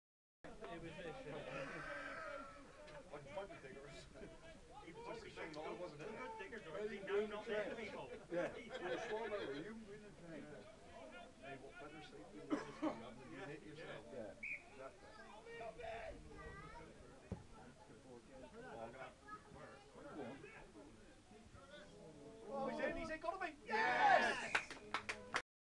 C.C,F.A Bond Timber Junior Cup 3RD Round Game, v St Stephen 10 v 0 Halsetown
match highlights Jay Davie takes a free-kick into the box and at the 2nd attempt Craig Coad smashes the 8th goal home.